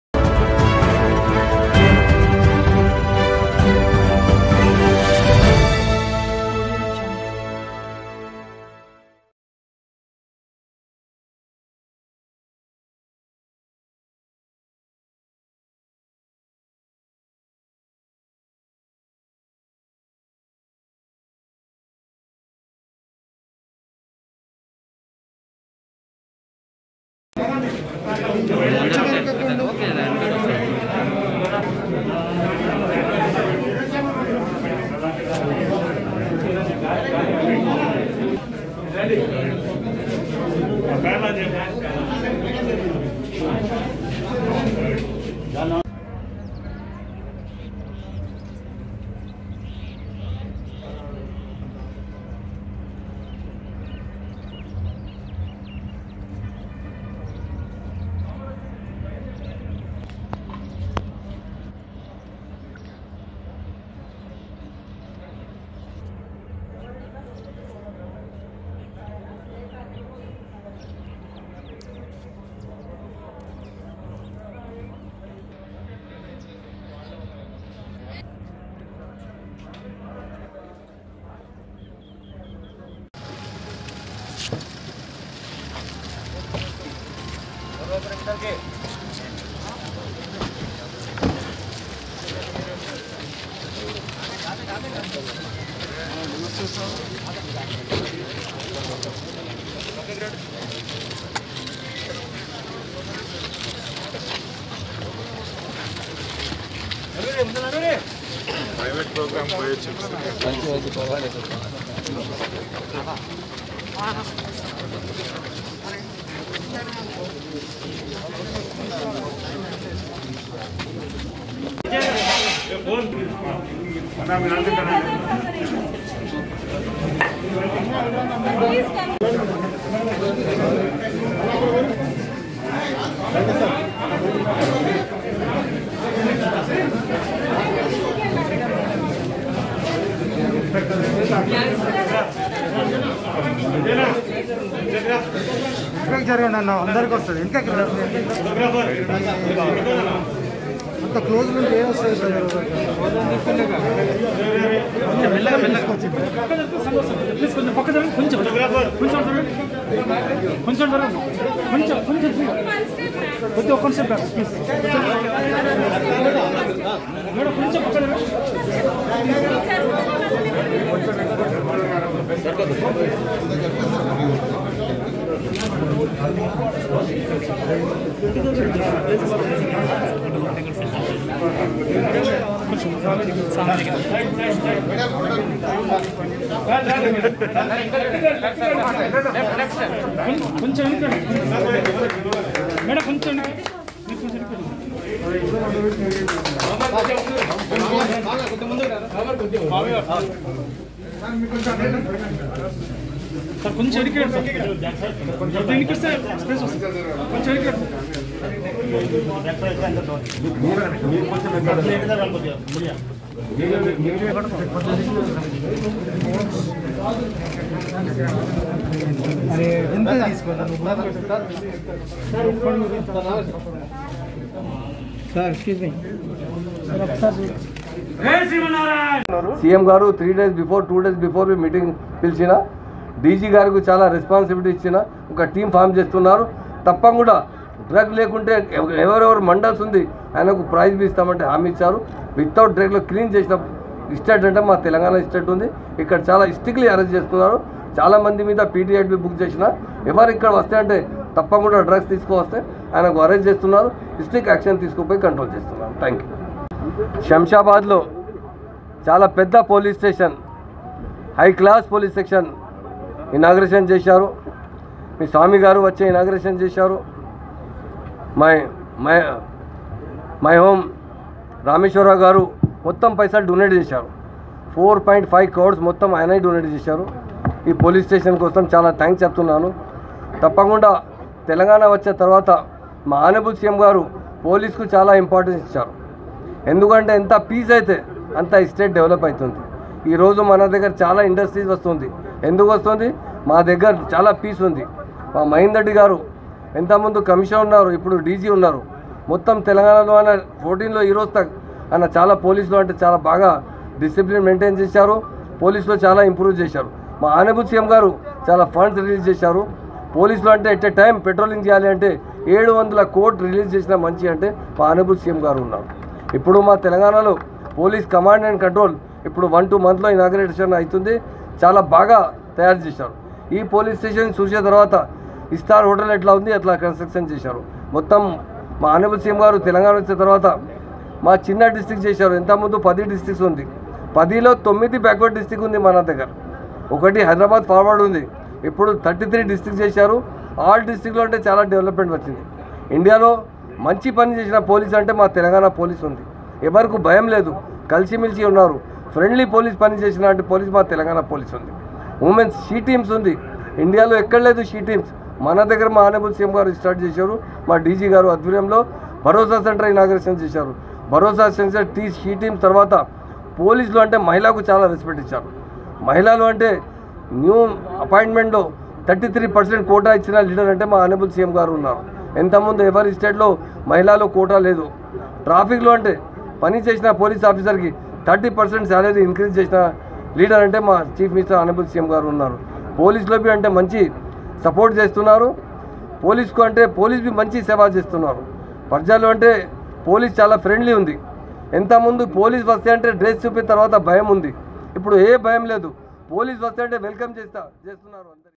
Inauguration of New Police Station of Shamshabad by Honorable Home Minister Sri. Mohammed Mahmood
Ali, Education Minister Smt. Sabitha Indira Reddy, DGP Sri. M. Mahendra Reddy IPS, CP. Sri. M. Stephen Raveendra IPS, ZP. Chairperson Smt. Anita Harinath Reddy, MP. Sri. Rajnath Reddy, MLC Sri. Mahender Reddy, MLC Smt. Vani Devi, MLA Sri. T. Prakash Goud will attend the Ceremony.